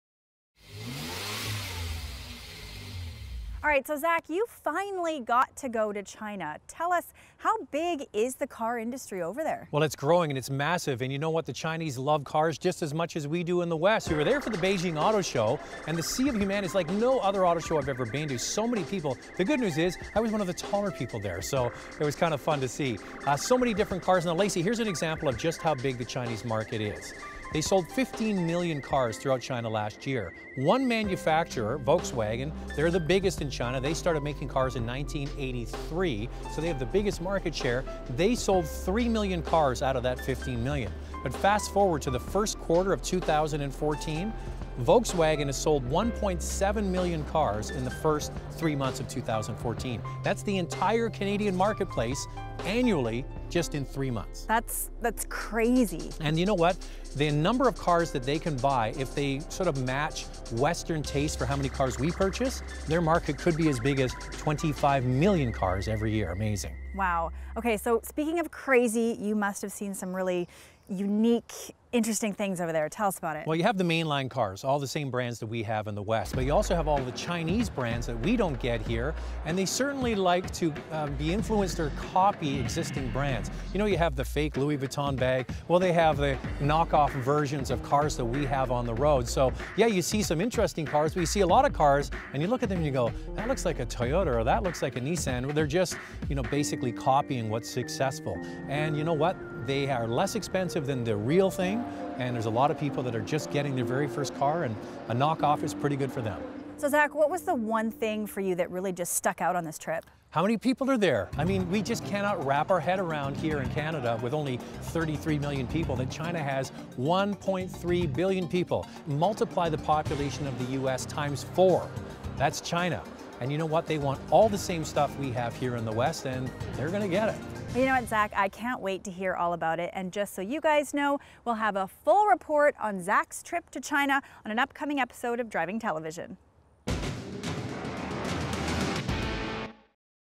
Chinese Car industry
The automotive industry in China has been the largest in the world measured by automobile unit production since 2008.[1][2][3] Since 2009 annual production of automobiles in China exceeds that of the European Union or that of the United States and Ja